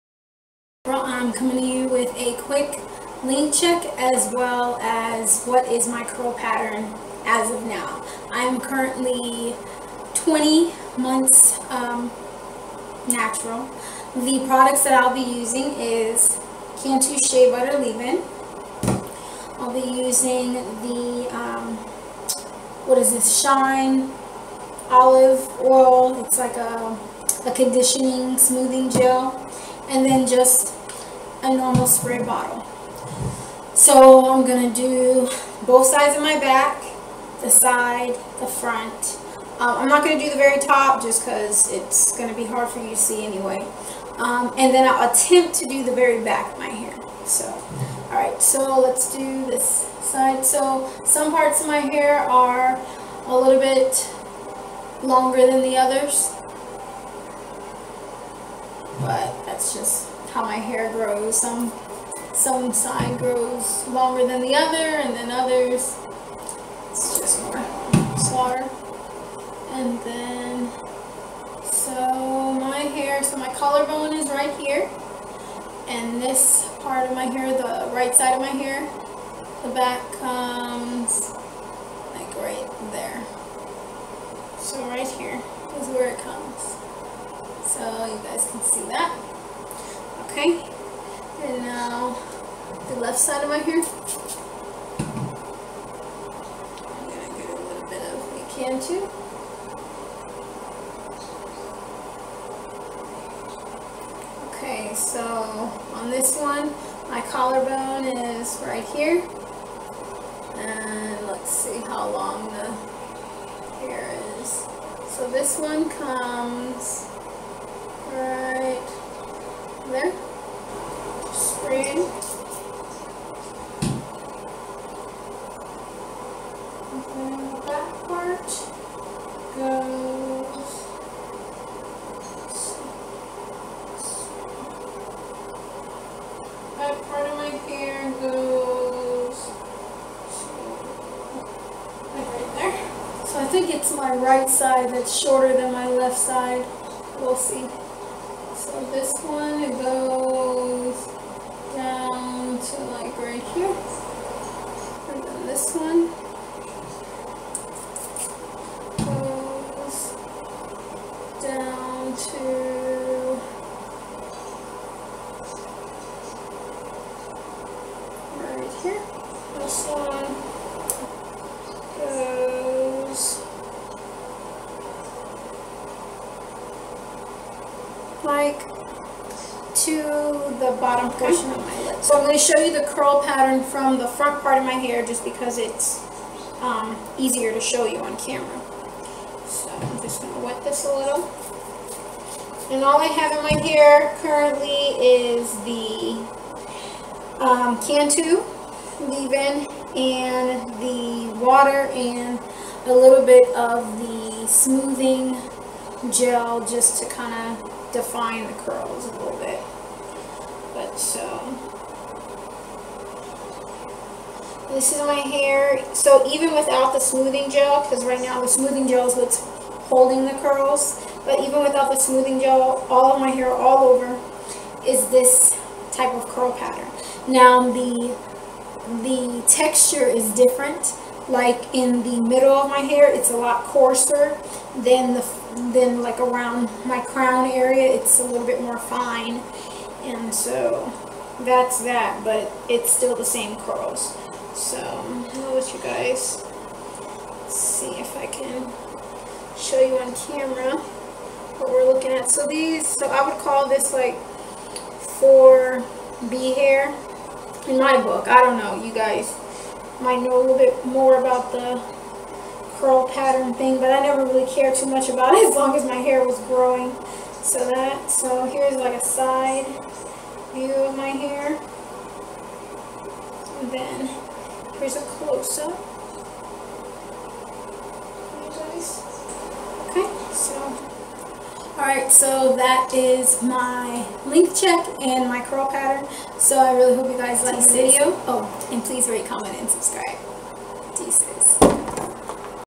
20 Month Length Check
Hey YouTubbers,
Here is my most recent length check. My growth is a reminder to myself why going natural was probably one of the best things I could have done for myself. Enjoy and good luck in your natural hair journey! Xoxo. Deuces